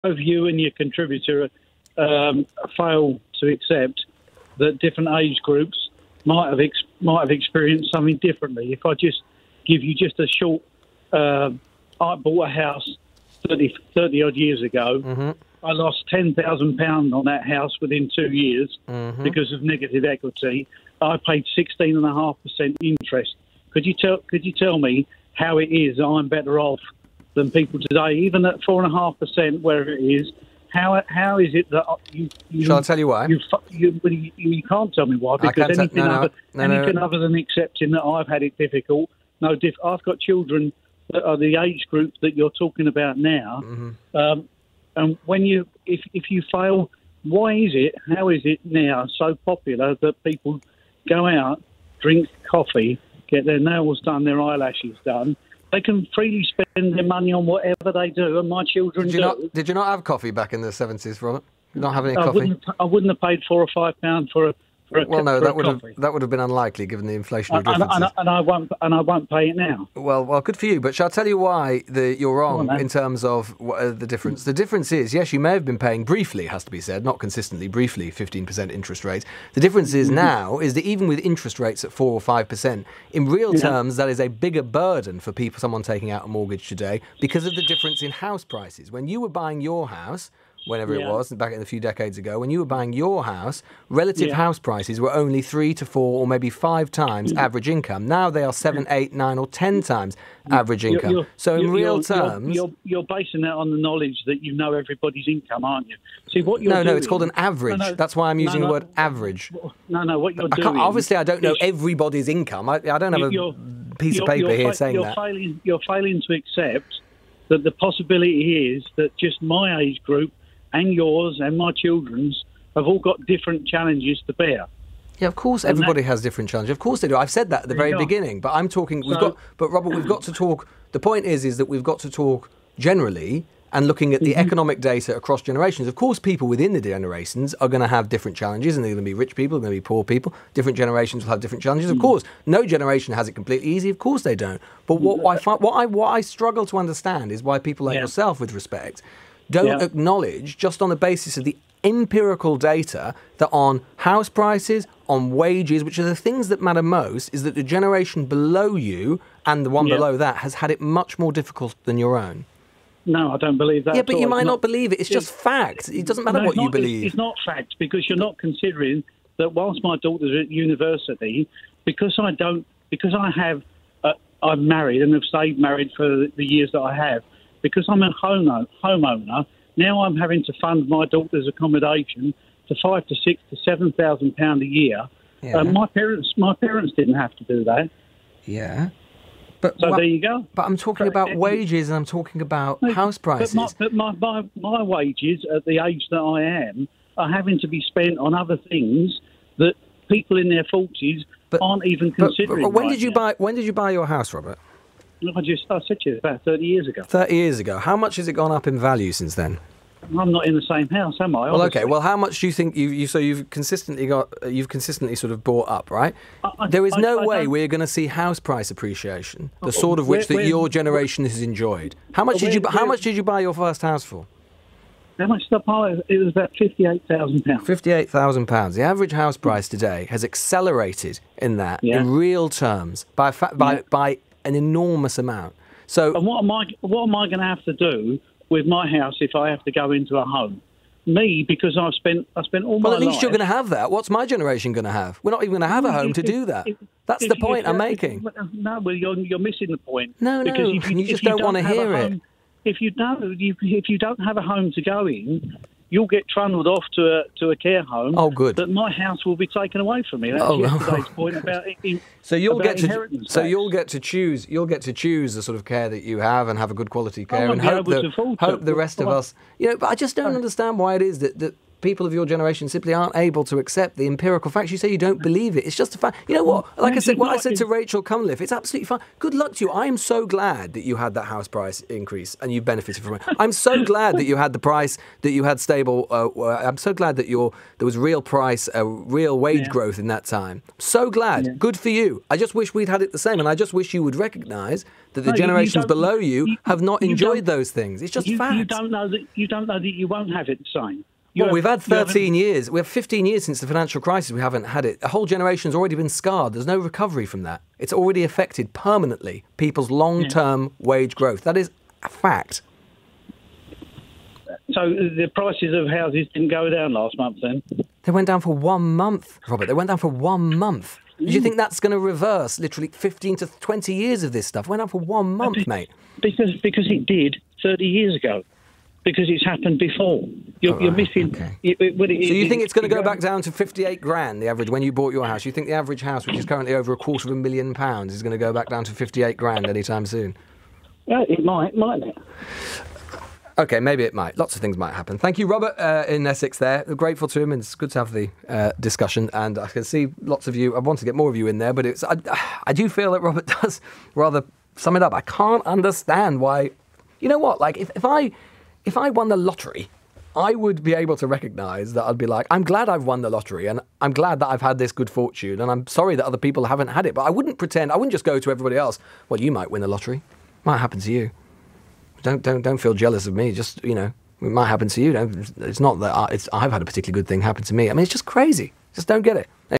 LBC caller rejects view that millennials are worse off than previous generation
'You're missing the point.'

'No, you just don't want to hear it!'

Lewis Goodall and this caller debate whether millennials are worse off than the generation before them.